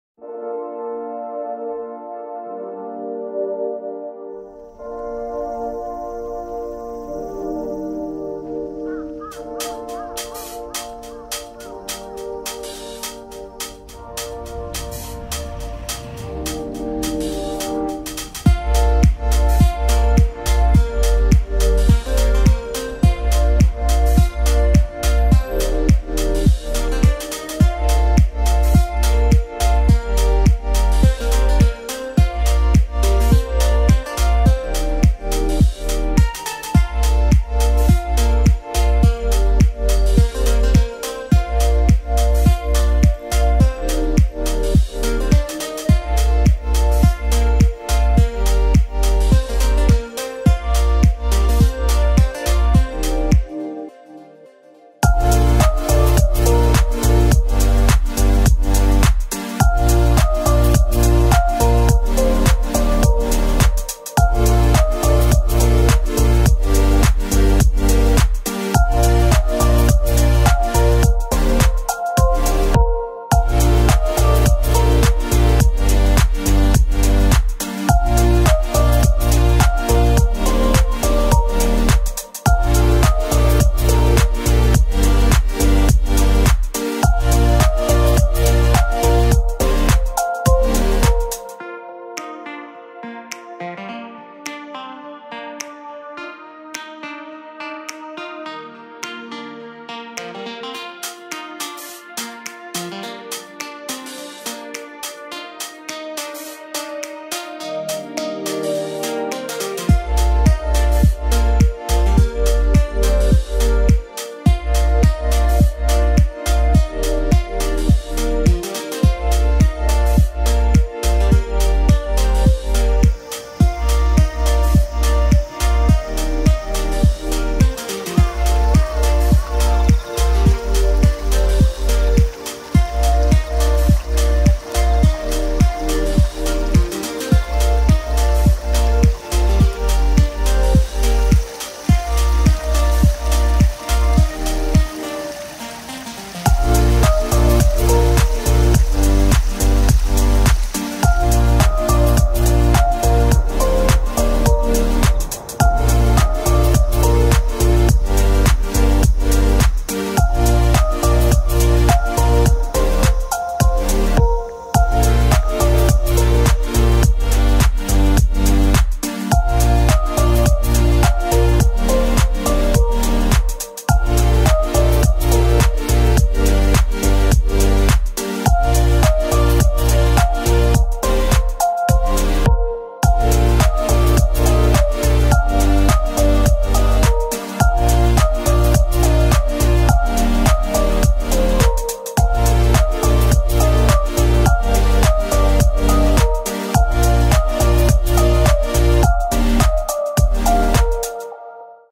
[VLOG]【4K】藤沢 小出川（川の土手の彼岸花）2022
藤沢市 小出川の土手約3Kmにわたって彼岸花が咲きます。
この日(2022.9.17)の彼岸花はまだ蕾が多かったですが、
彼岸花祭りが催される9月24日には満開を迎えるでしょう。
（小出川：湘南台駅西口よりバスで 慶応大学下車 徒歩10分）